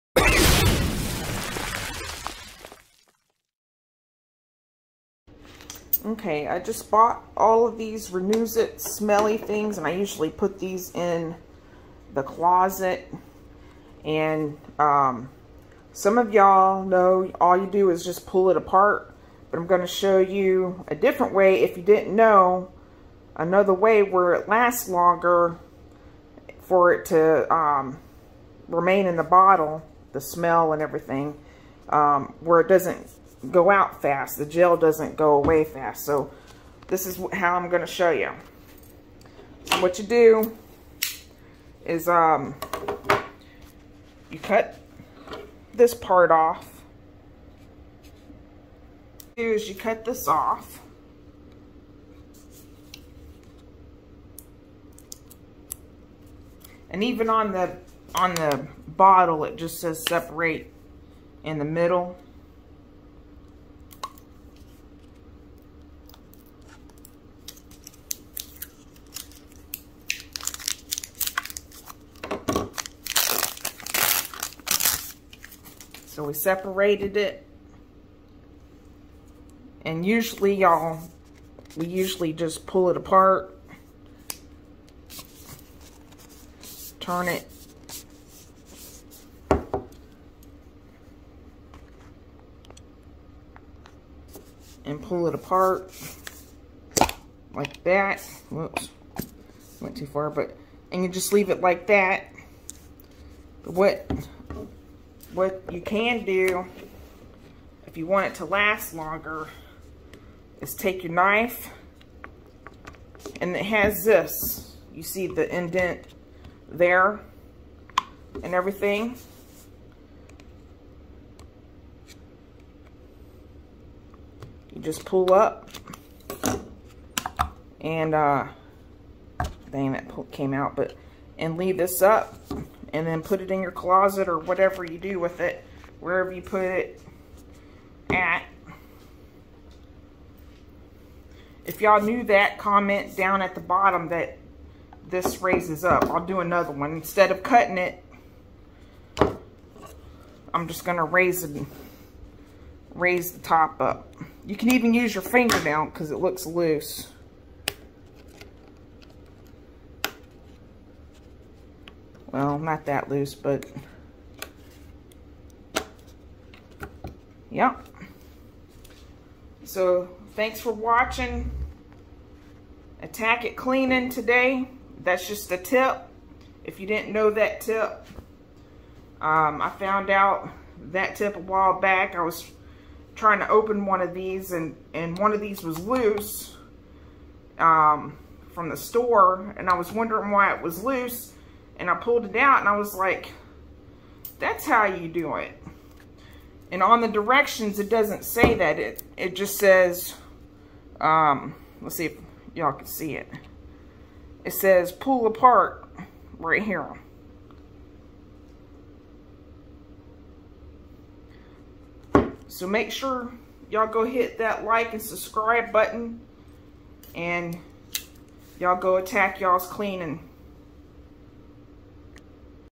DO YOU KNOW THIS
Showing a different way on opening fragrance container to make it last longer.